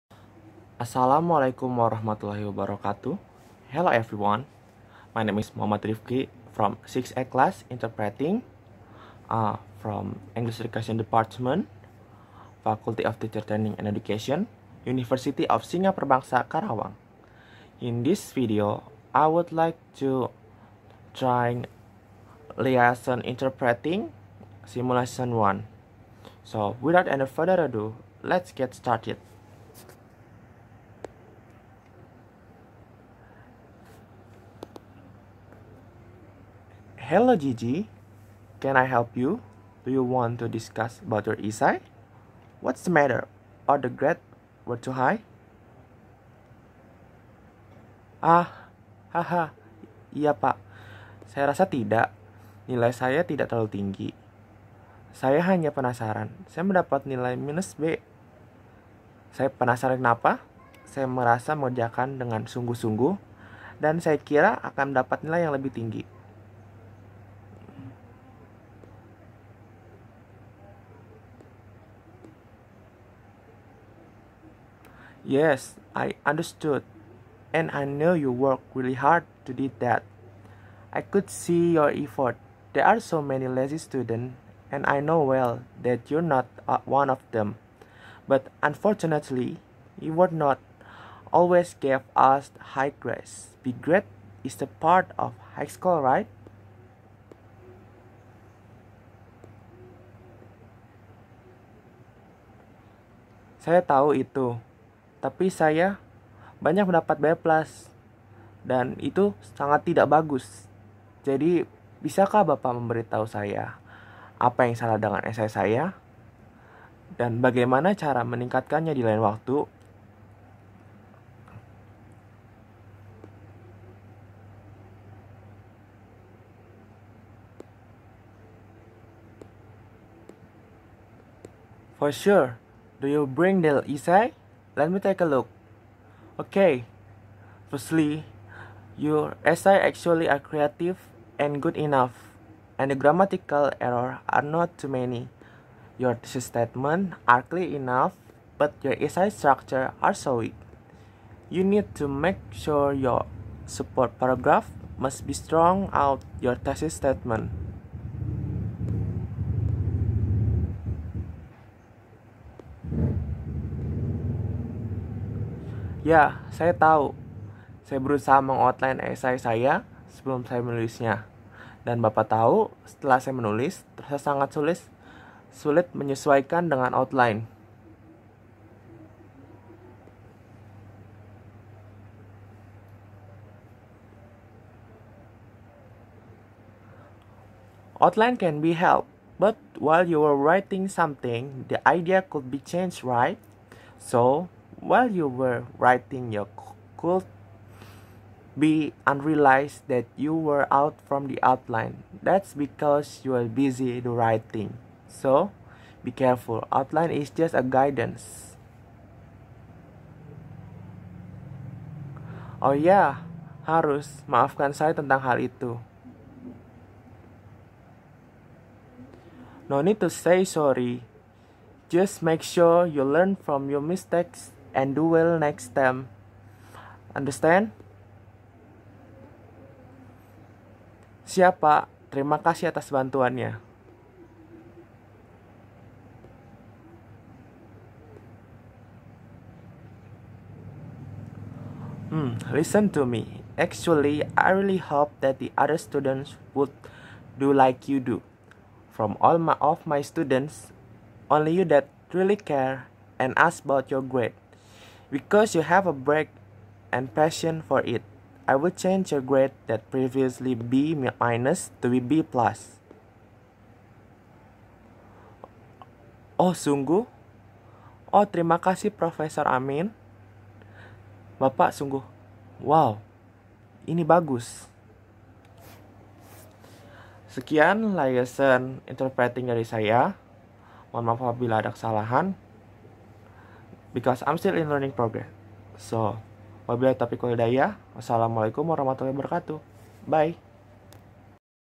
Liaison Interpreting "Simulation 1" Muhammad Rifki UNSIKA Tahun 2021
Name : Muhammad Rifki
Class : 6A Interpreting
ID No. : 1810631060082
Lecturer : Putri Kamalia Hakim, S. Pd., M. Hum